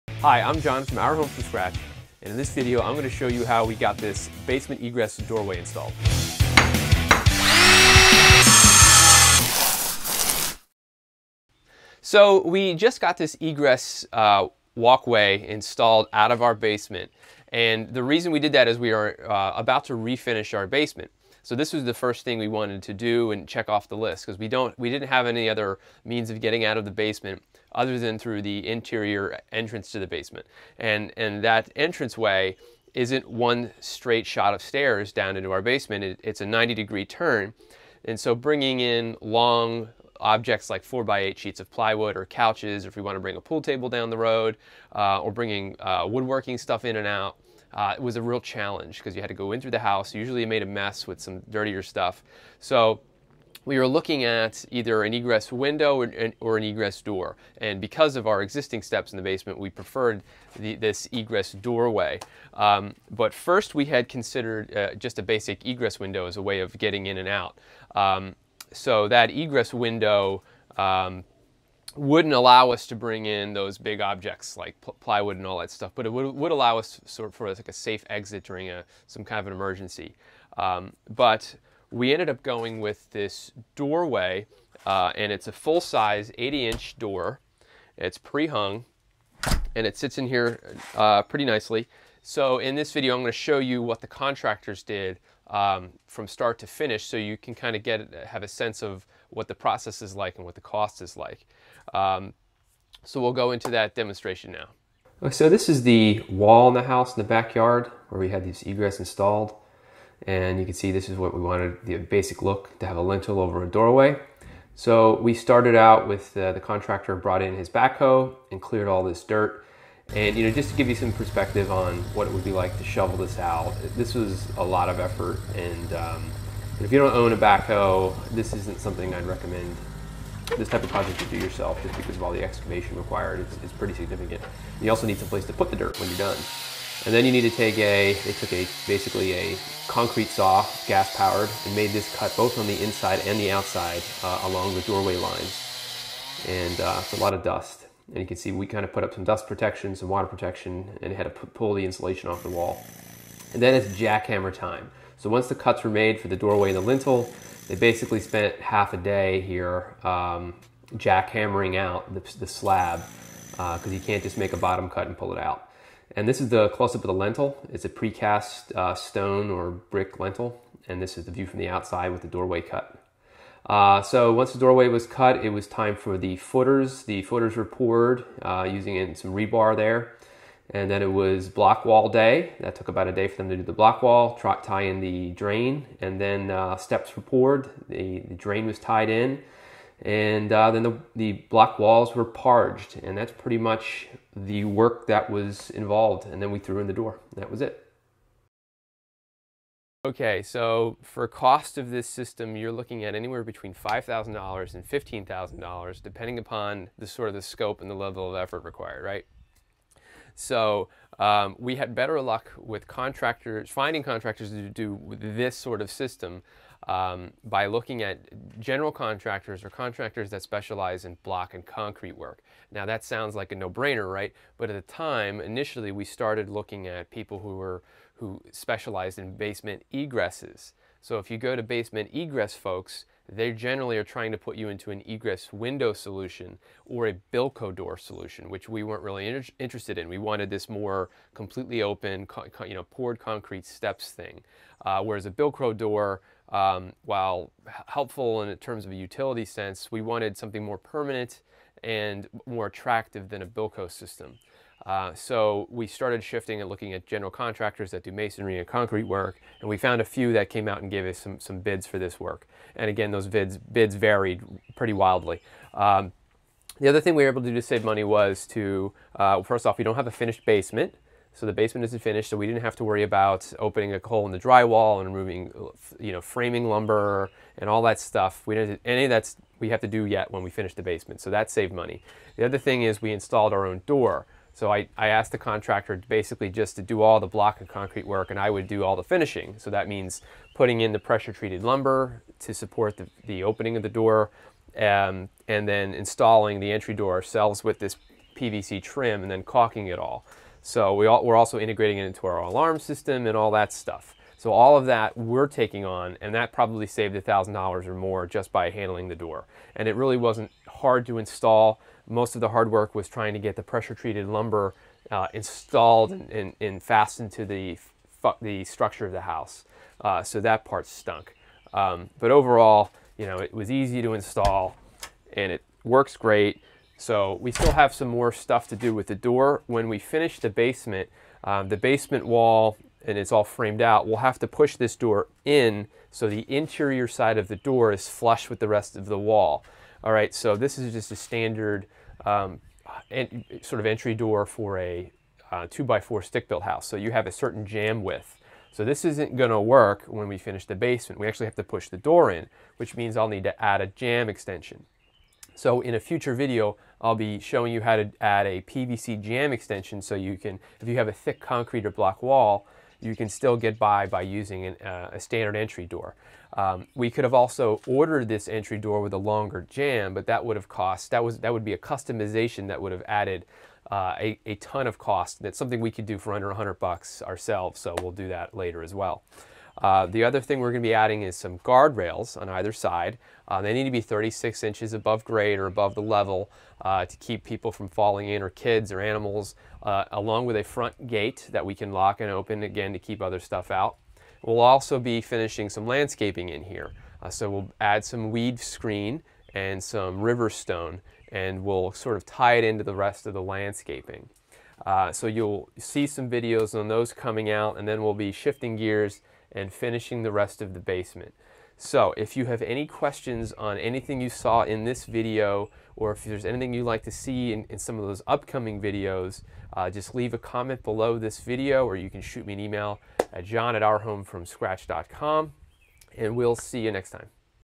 How to Install a Basement Egress
In this video, John from Our Home from Scratch demonstrates the process involved with installing a basement egress.

FYI:  I don't show you how to install the door.  That's a simple process with some 2x4 pressure treated lumber screwed directly into the concrete opening.  The pre-hung entry door is then screwed into the pressure treated lumber.